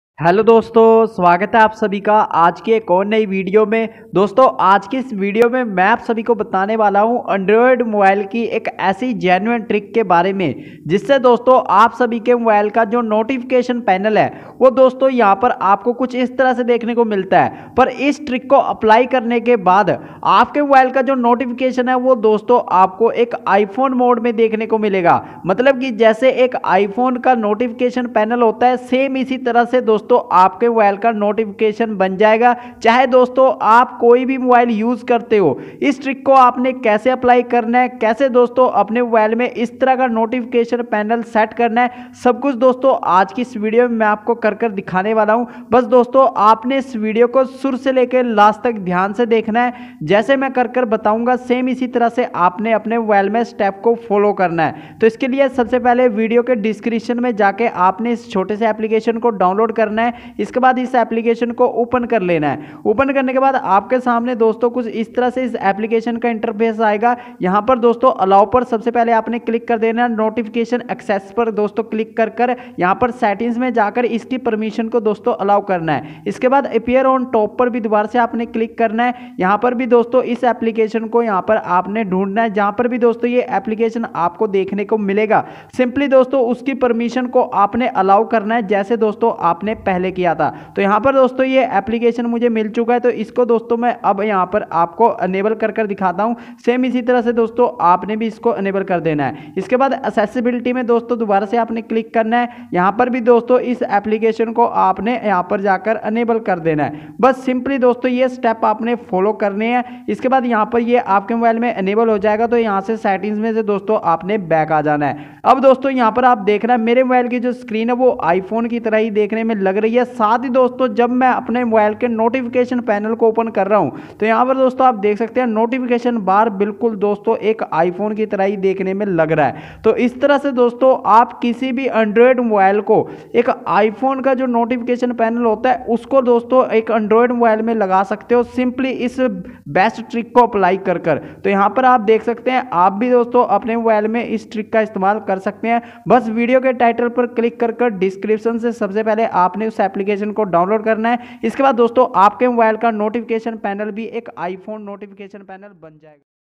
Change Notification pannel android to iphone l फोन में iphone वाला notification bar कैसे लगाए
Change Notification pannel android to iphone l फोन में iphone वाला notification bar कैसे लगाए l